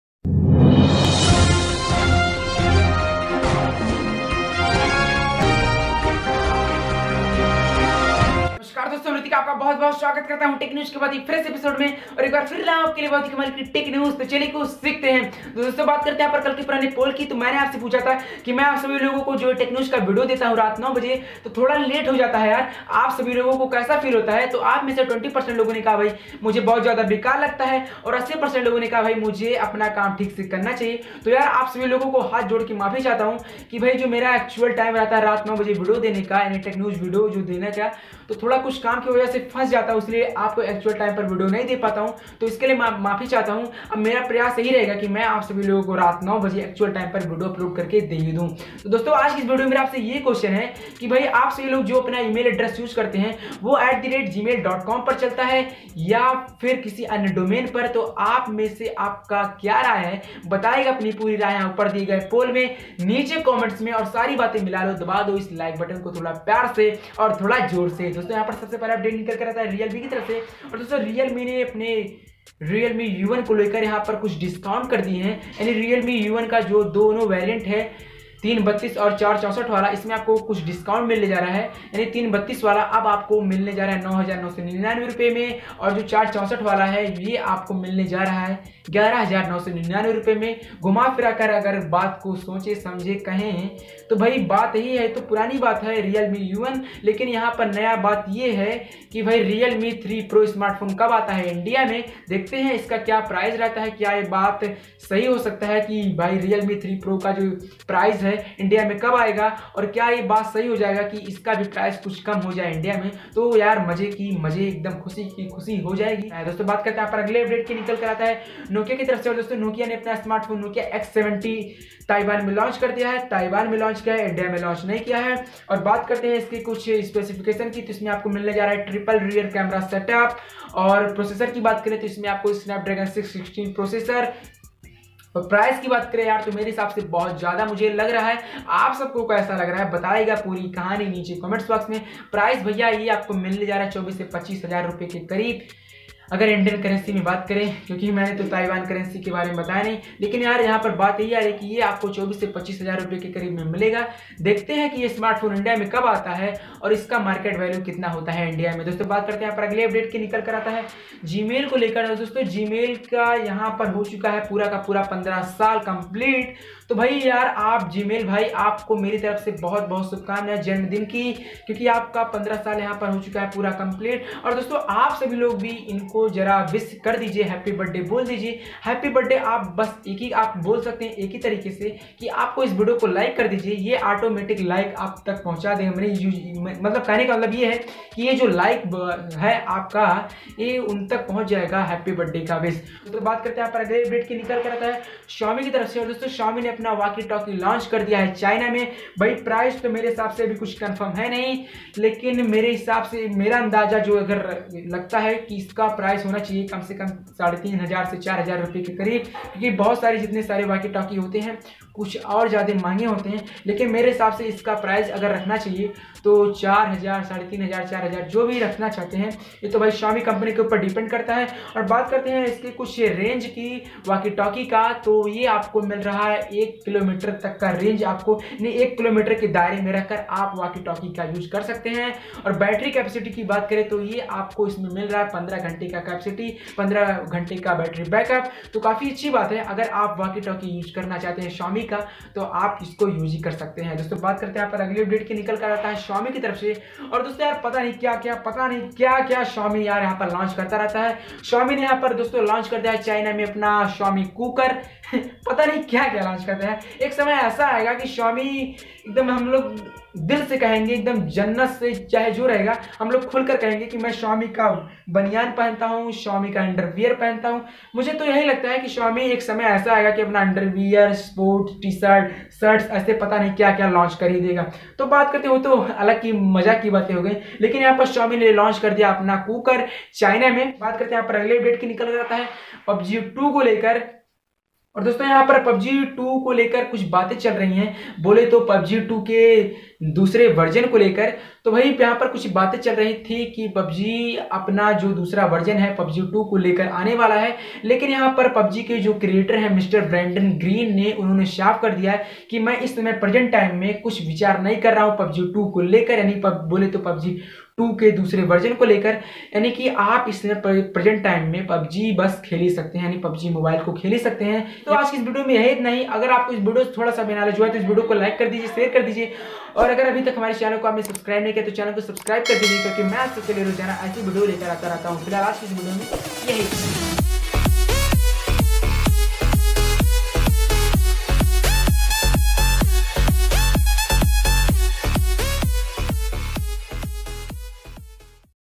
Tech News #10- Realme U1, PUBG Mobile, NOKIA X71, Gmail 15 years, Xiaomi Cookar
Namaskar dosto tech news ke is episode mein maine aapse kuch majedar tech News share ki hai jaise Realme U1 Discount, PUBG mobile 2, Nokia X71 launch, Gmail celebration, Xiaomi Cooker Xiaomi Walkie Talkie aur bahut kuch mujhe umeed hai ki aapko ye video pasand aayegi...

My Gears-
Selfie stick